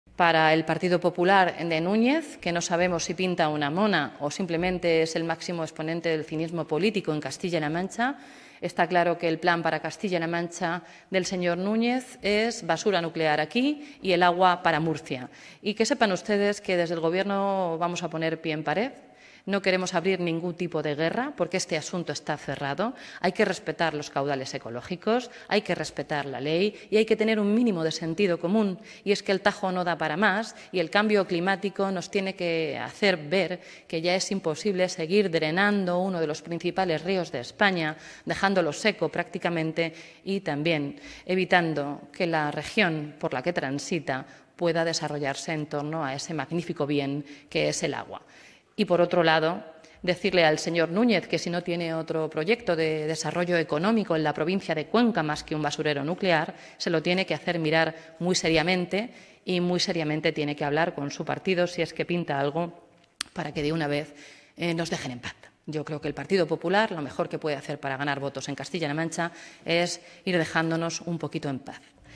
Junta critica el "cinismo" del PP por apoyar la reactivación del ATC
La consejera de Igualdad y portavoz del Gobierno regional, Blanca Fernández, ha criticado el "cinismo" del presidente del PP castellanomanchego, Paco Núñez, después de que el Grupo Popular en el Congreso de los Diputados haya presentado entre sus enmiendas a los Presupuestos Generales del Estado (PGE) una para reactivar el proyecto de Almacén Temporal Centralizado de residuos nucleares en la localidad conquense de Villar de Cañas y ha pedido a los 'populares' que dejen "en paz" a la región.

(Fuente: Gobierno Castilla-La Mancha)